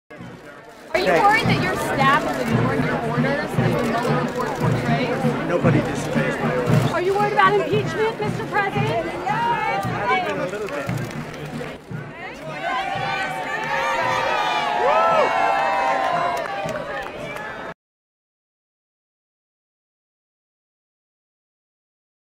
ترامب يهدد الكونجرس باللجوء للمحكمة الدستورية لوقف محاولات عزله
-هدد الرئيس الأمريكي دونالد ترامب الكونجرس باللجوء للمحكمة الدستورية العليا لموقف محاولات عزله

- المعسكر الديمقراطي في مجلس النواب يستعد لإدانة الرئيس على خلفية نشر تقرير مولر حول تدخل روسيا في الانتخابات